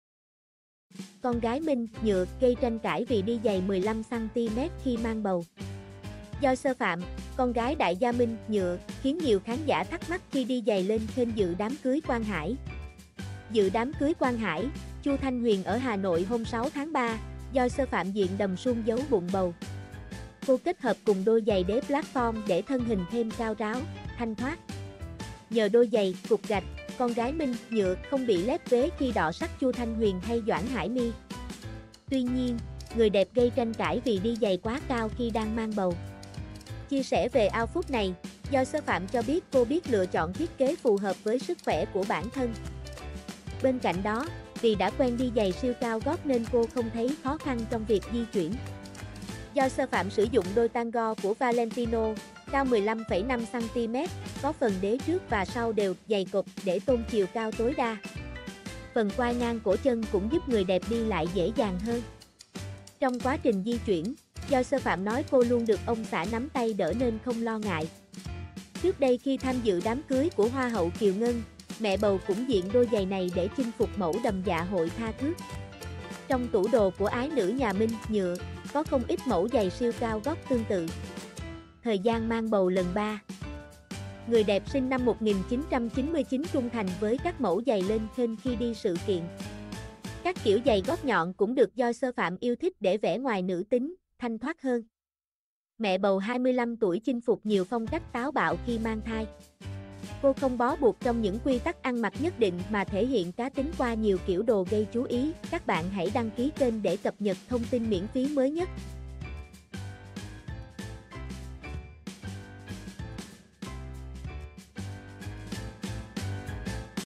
Con gái Minh 'Nhựa' gây tranh cãi vì đi giày 15 cm khi mang bầu
Joyce Phạm - con gái đại gia Minh 'Nhựa' - khiến nhiều khán giả thắc mắc khi đi giày lênh khênh dự đám cưới Quang Hải.

Dự đám cưới Quang Hải - Chu Thanh Huyền ở Hà Nội hôm 6/3, Joyce Phạm diện đầm suông giấu bụng bầu. Cô kết hợp cùng đôi giày đế platform để thân hình thêm cao ráo, thanh thoát.

Nhờ đôi giày 'cục gạch', con gái Minh 'Nhựa' không bị lép vế khi đọ sắc Chu Thanh Huyền hay Doãn Hải My. Tuy nhiên, người đẹp gây tranh cãi vì đi giày quá cao khi đang mang bầu. Chia sẻ về outfit này, Joyce Phạm cho biết cô biết lựa chọn thiết kế phù hợp với sức khỏe của bản thân. Bên cạnh đó, vì đã quen đi giày siêu cao gót nên cô không thấy khó khăn trong việc di chuyển.

Joyce Phạm sử dụng đôi Tan-go của Valentino, cao 15,5 cm, có phần đế trước và sau đều 'dày cộp' để tôn chiều cao tối đa. Phần quai ngang cổ chân cũng giúp người đẹp đi lại dễ dàng hơn. Trong quá trình di chuyển, Joyce Phạm nói cô luôn được ông xã nắm tay đỡ nên không lo ngại.

Trước đây khi tham dự đám cưới của hoa hậu Kiều Ngân, mẹ bầu cũng diện đôi giày này để chinh phục mẫu đầm dạ hội tha thướt.

Trong tủ đồ của ái nữ nhà Minh 'Nhựa', có không ít mẫu giày siêu cao gót tương tự. Thời gian mang bầu lần ba, người đẹp sinh năm 1999 trung thành với các mẫu giày lênh khênh khi đi sự kiện.

Các kiểu giày gót nhọn cũng được Joyce Phạm yêu thích để vẻ ngoài nữ tính, thanh thoát hơn.

Mẹ bầu 25 tuổi chinh phục nhiều phong cách táo bạo khi mang thai. Cô không bó buộc trong những quy tắc ăn mặc nhất định mà thể hiện cá tính qua nhiều kiểu đồ gây chú ý.